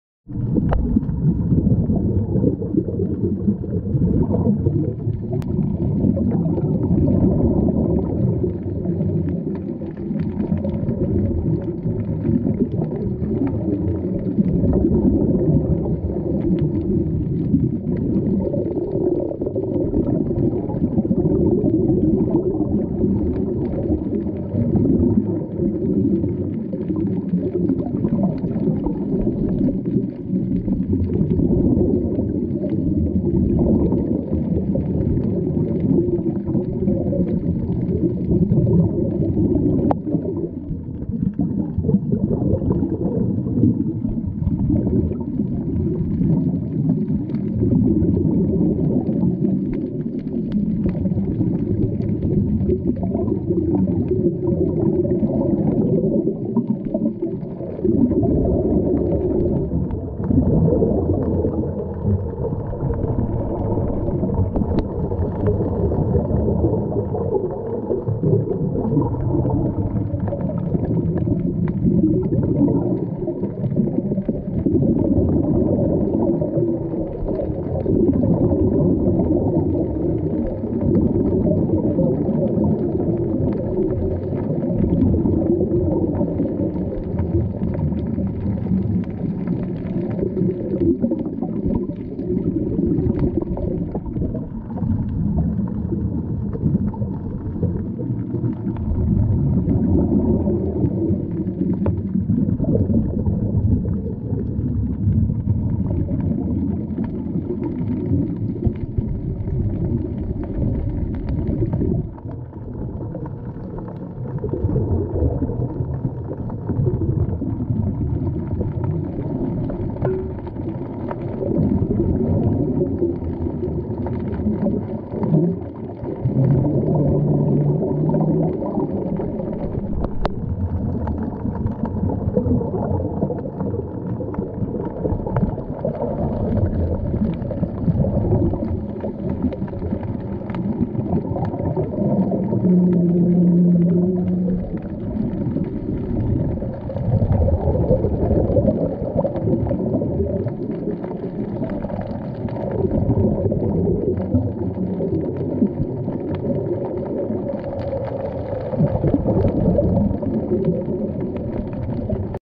Scuba Diving at Tarkarli Beach | Underwater Diving | Priyanka
Scuba Diving at Tarkarli Beach
Underwater Diving in Arebian Sea
Scuba Diver: Priyanka Suhas Bachhav